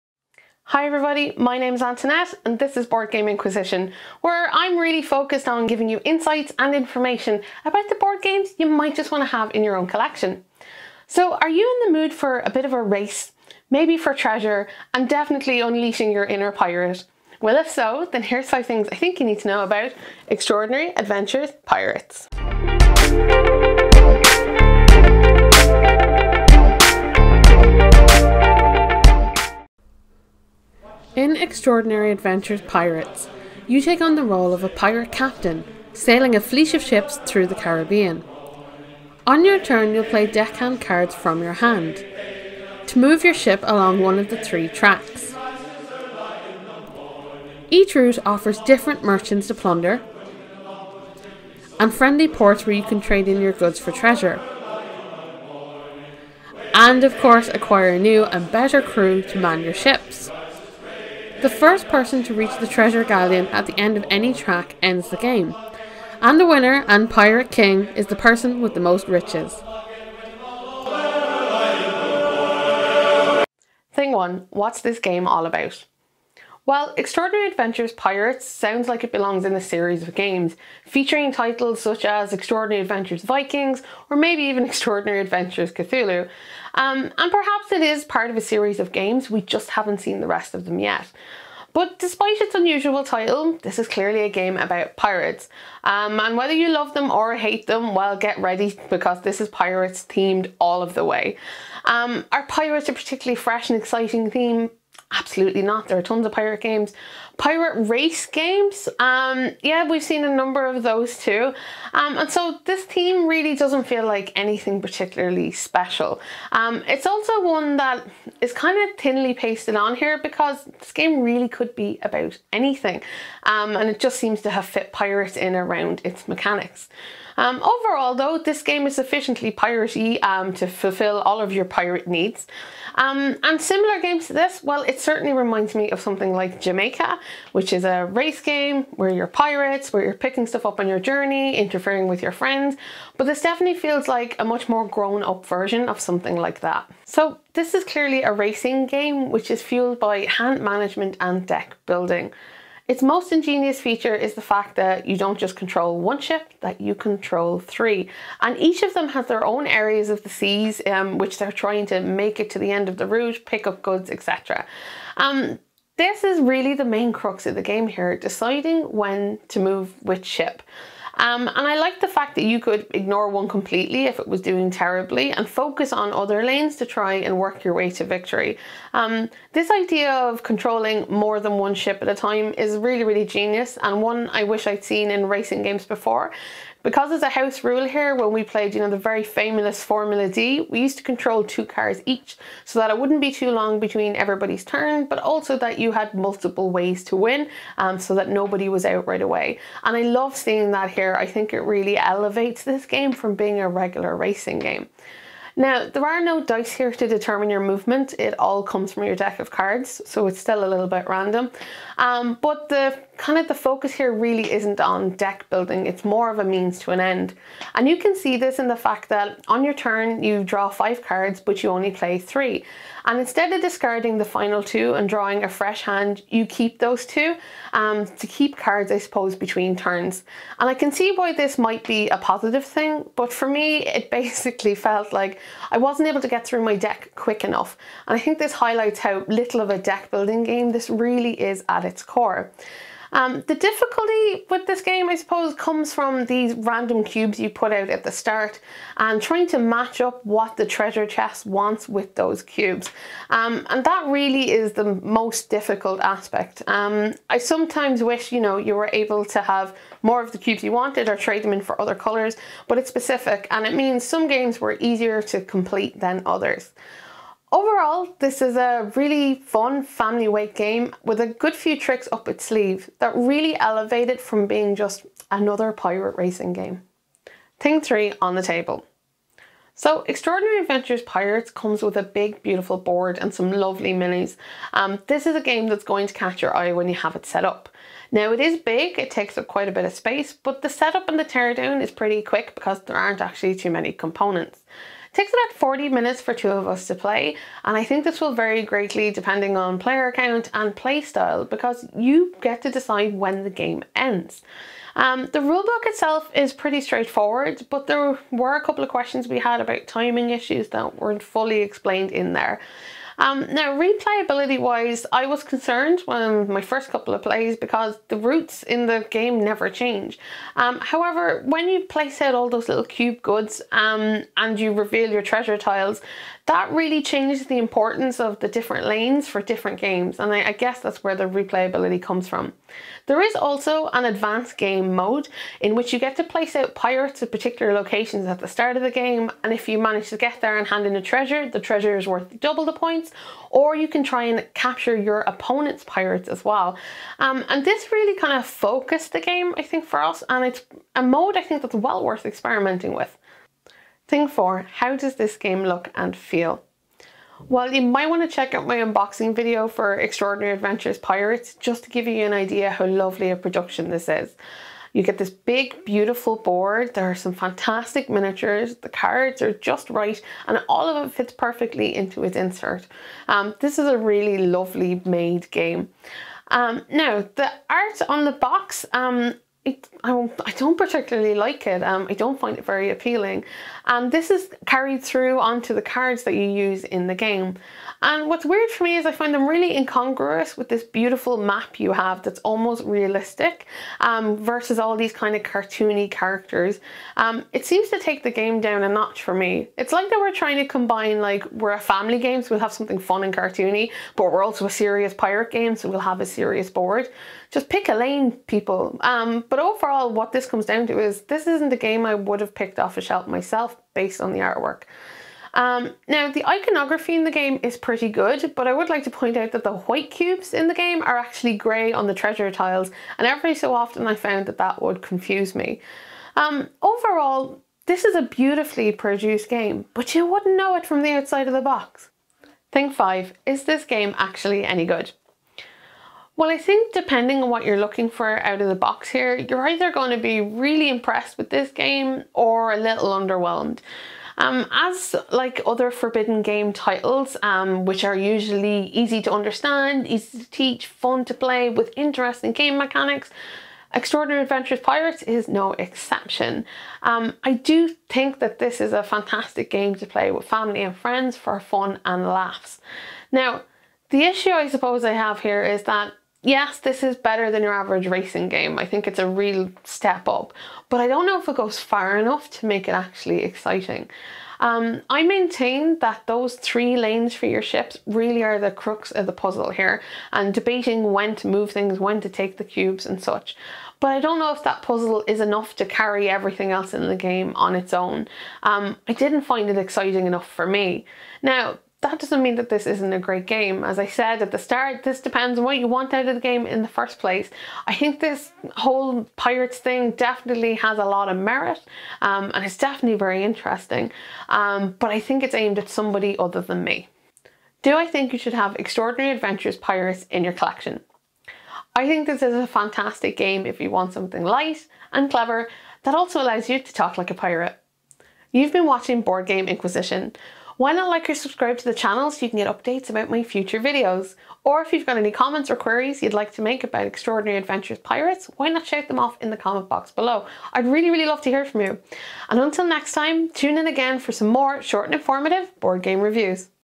5 Things You Need To Know About Extraordinary Adventures: Pirates
About This Video: In this video review, Board Game Inquisition reviews what we feel are the five most important aspects of the board game Extraordinary Adventures: Pirates published by Forbidden Games and designed by Don Beyer and Glenn Drover. We discuss the topic of the game, the mechanics it uses, what the game is like on the table, how it looks and feels and of course it's pros and cons. Overall Extraordinary Adventures: Pirates is an interesting take on your traditional racing game!